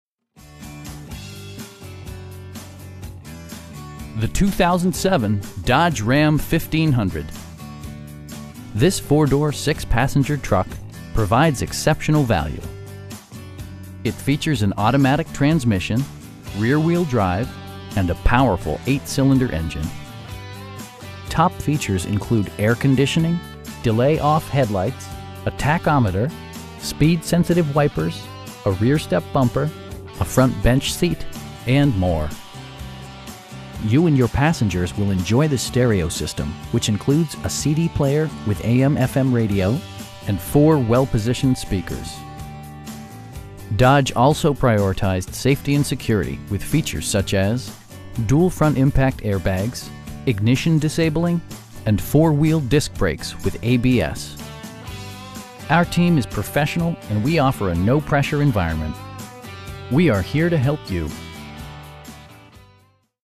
2007 Dodge Ram 1500 in North Richland Hills, TX 76180
Huggins Honda
7551 NE Loop 820

The 2007 Dodge Ram 1500. This 4 door, 6 passenger truck provides exceptional value! It features an automatic transmission, rear-wheel drive, and a powerful 8 cylinder engine. Top features include air conditioning, delay-off headlights, a tachometer, speed sensitive wipers, a rear step bumper, a front bench seat, tilt steering wheel, and more. You and your passengers will enjoy the stereo system, which includes a CD player with AM/FM radio, and 4 speakers, providing excellent sound throughout the cabin. Dodge also prioritized safety and security with features such as: dual front impact airbags, ignition disabling, and 4 wheel disc brakes with ABS. Our team is professional, and we offer a no-pressure environment. We'd be happy to answer any questions that you may have. We are here to help you.